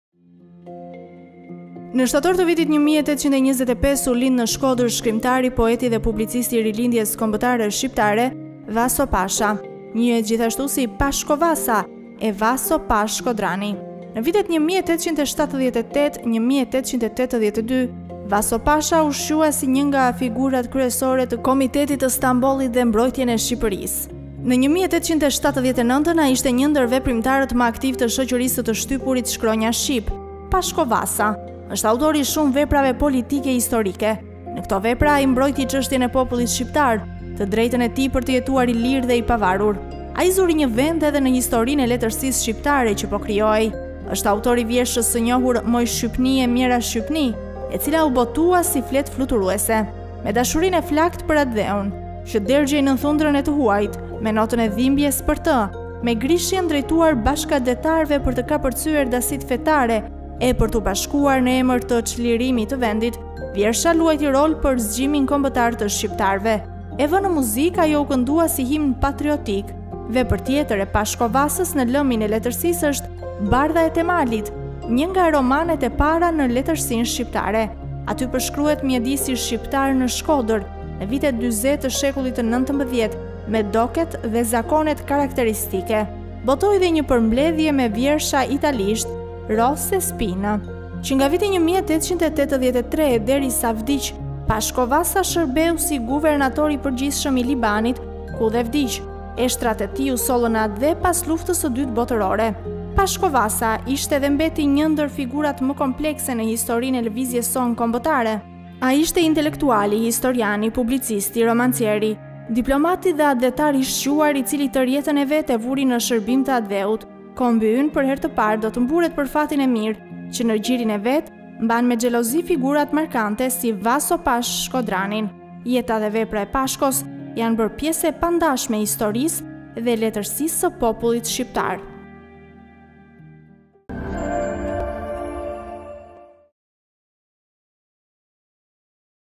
Pashko Vasa, jeta dhe vepra
Në  shtator te vitit  1825 u lind në Shkodër shkrimtari, poeti dhe publicisti i Rilindjes Kombëtare Shqiptare, Vaso Pasha. Njihet gjithashtu si Pashko Vasa e Vaso pashë Shkodrani.
Në vitet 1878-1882 V.Pasha u shqua si një nga figurat kryesore të Komitetit të Stambollit për mbrojtjen e Shqipërisë. Më 1879 ai ishte një ndër veprimtarët më aktivë të “Shoqërisë së të shtypurit shkronja shqip”.